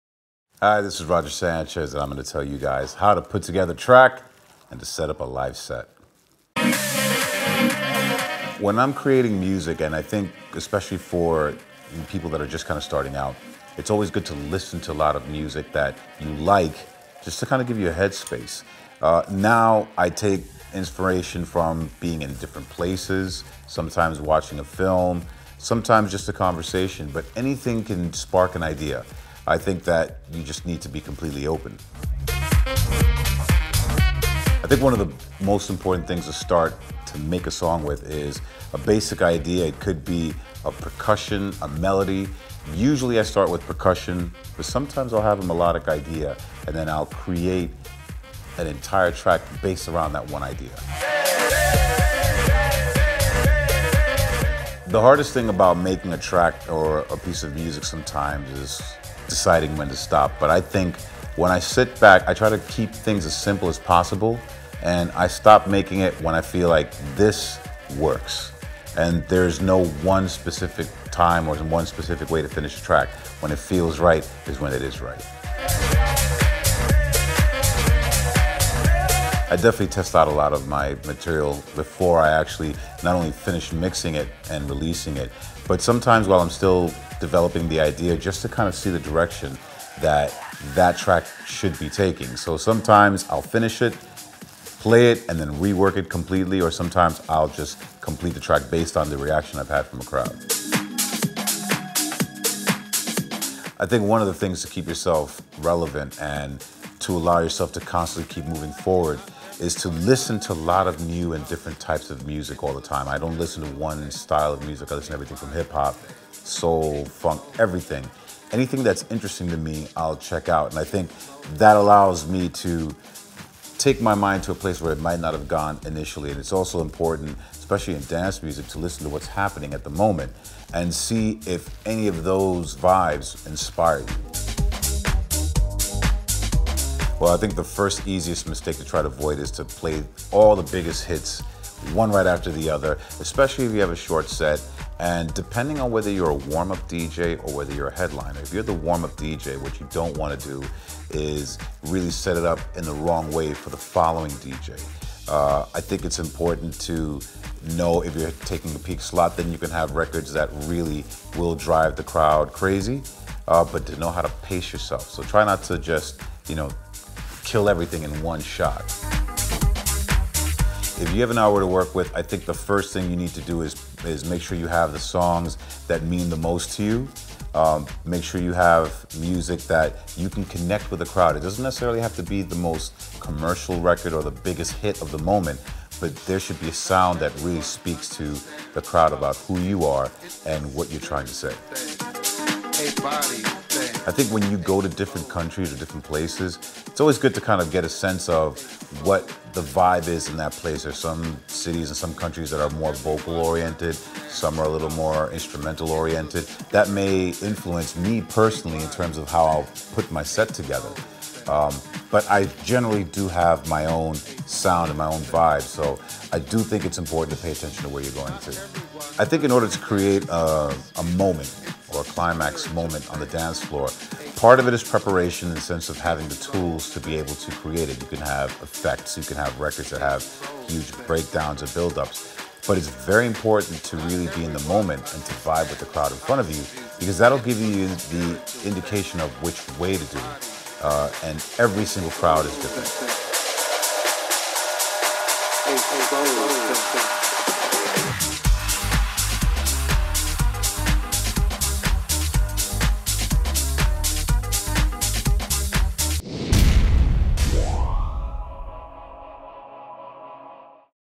Roger Sanchez's Tips For DJs
World renowned DJ Roger Sanchez gives his tips on how to be a successful DJ, how to produce a song and how to put together a live set.